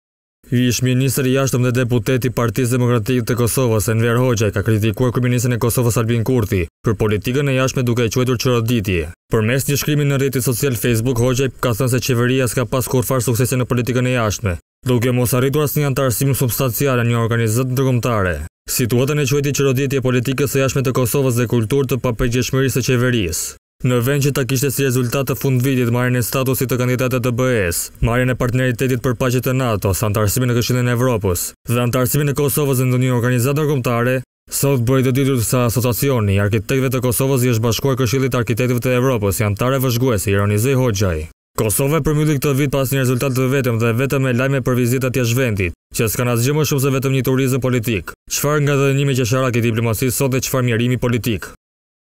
Enver Hoxhaj sulmon Kurtin/ “Ngadhënjimi” qesharak dhe mjerimi politik
Ish ministri i Jashtëm dhe deputeti i Partisë Demokratike të Kosovës, Enver Hoxhaj, ka kritikuar kryeministrin e Kosovës, Albin Kurti për politikën e jashtme duke e quajtur “çoroditje”.
Hoxhaj, përmes një shkrimi në rrjetin social ‘Facebook’ ka thënë Qeveria s’ka pasur kurrfarë suksesi në politikën e jashtme duke mos arritur asnjë anëtarësim substancial në ndonjë organizëm ndërkombëtar.
Situatën e quajti “çoroditje e politikës së jashtme të Kosovës dhe kulturë të papërgjegjshmërisë së qeverisë”.

𝐀𝐮𝐭𝐨𝐫 : NTV